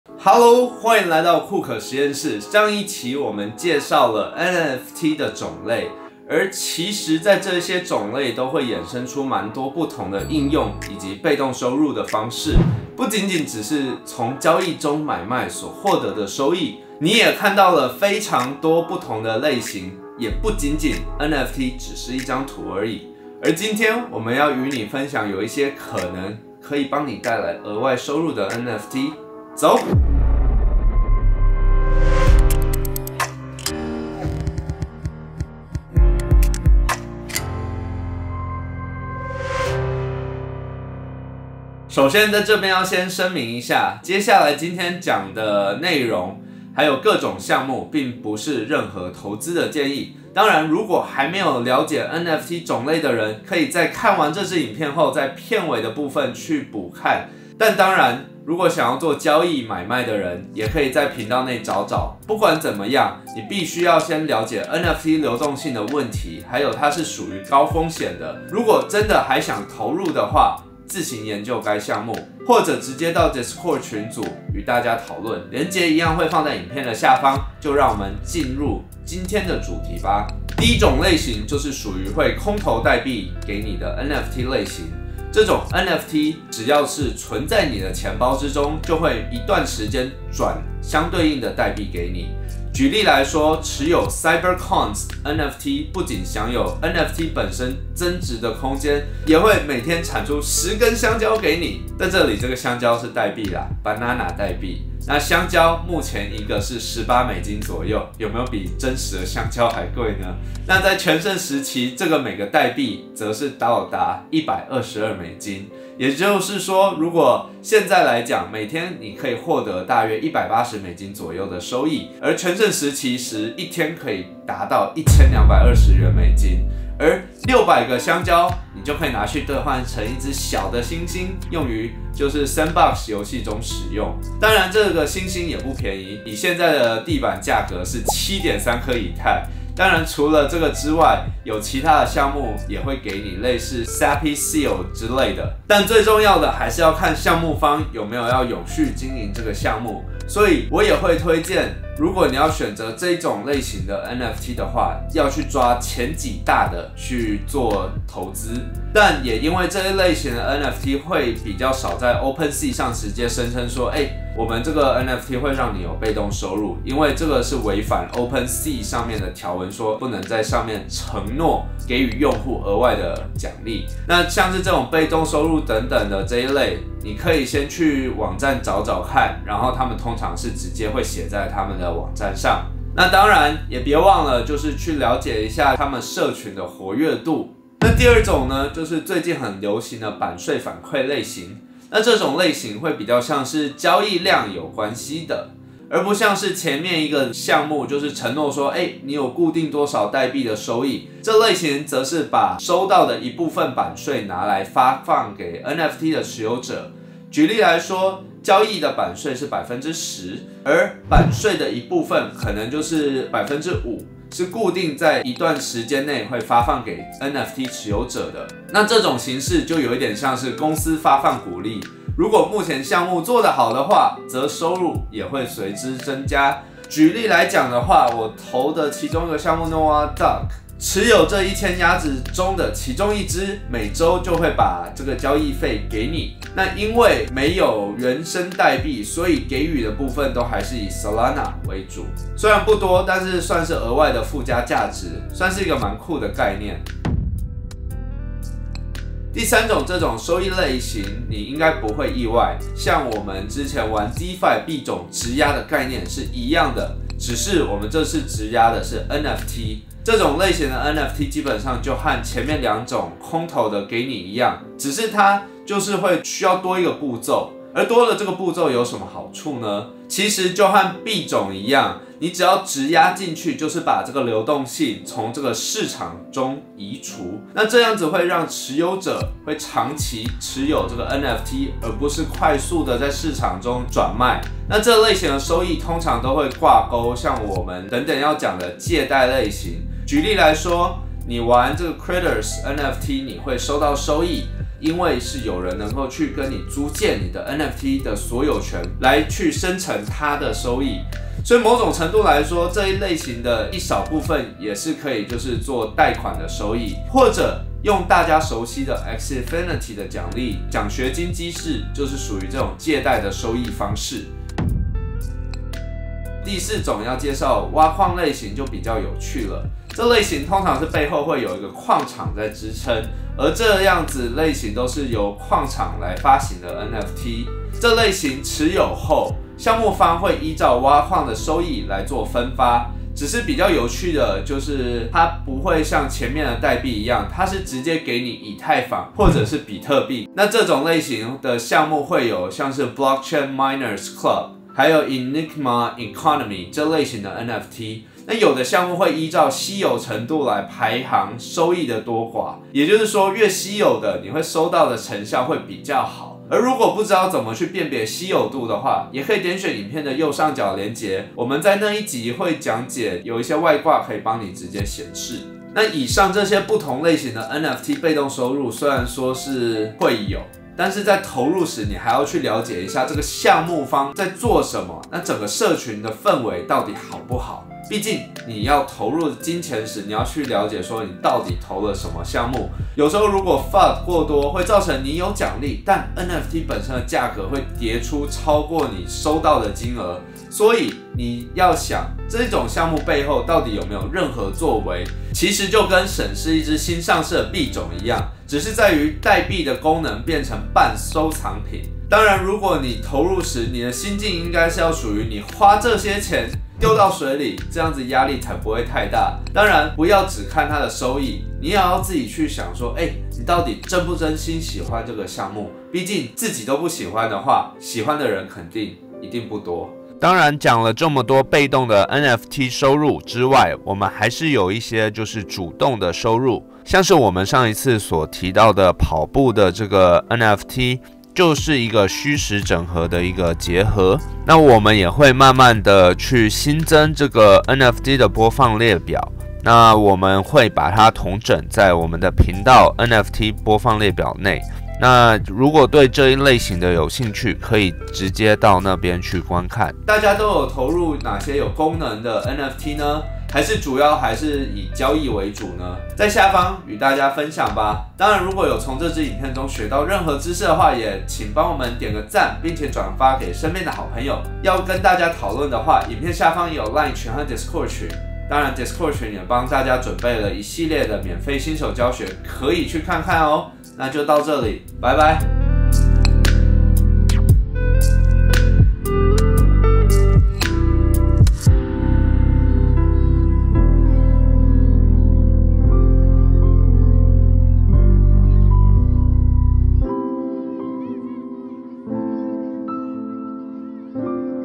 能為你帶來被動收入的NFT?每天給你帶來1000元的NFT？每天分香蕉？挖礦分潤？版稅抽成？
被動收益類型的NFT有很多種，其背後都運作不同的機制。有的是可以分潤挖礦收益，有的會空投香蕉給你，而有的會給你分潤交易所抽成的版稅，到底這些項目的背後都是怎麼運作呢？這集是帶你探討NFT的一些有趣被動賦能，不包含任何投資建議但可以去看看有什麼些有趣的項目！

▬▬▬▬▬▬▬▬▬ 互動群組▬▬▬▬▬▬▬▬▬
【加入酷可實驗室會員】
可以使用電腦在影片下方會有Join按鈕
【酷可實驗室Discord群】
0:00 開頭
0:42 聲明
1:21 收益種類
7:30 總結